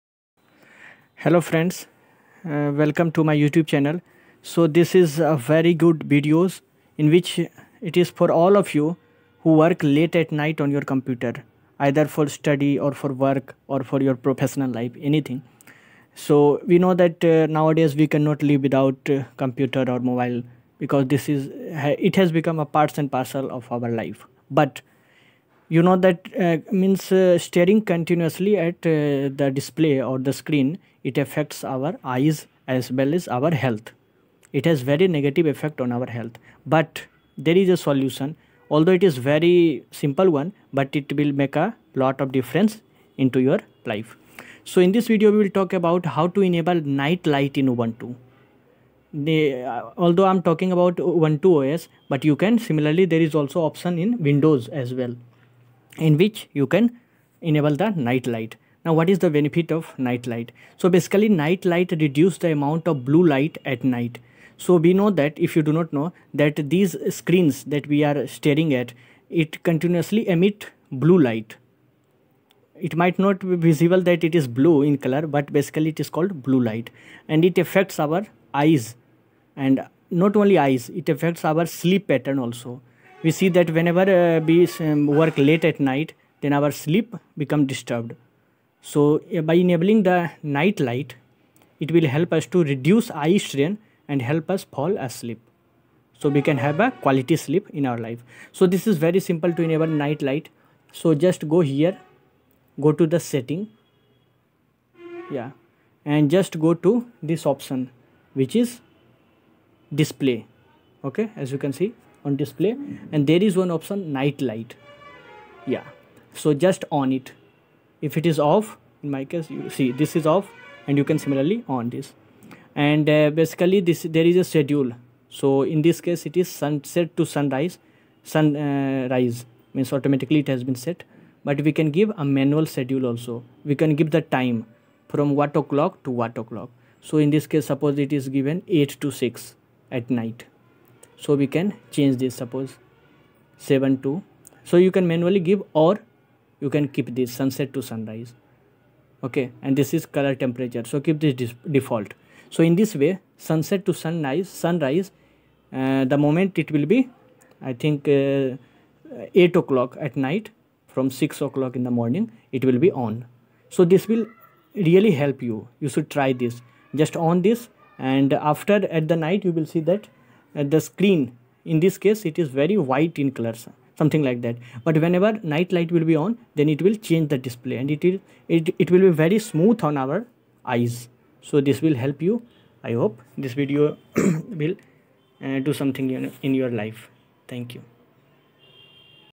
Enable Night light and protect your eyes from blue light in ubuntu.
This video will help you reduce the effect of blue light radiated from digital devices on our eyes and overall health.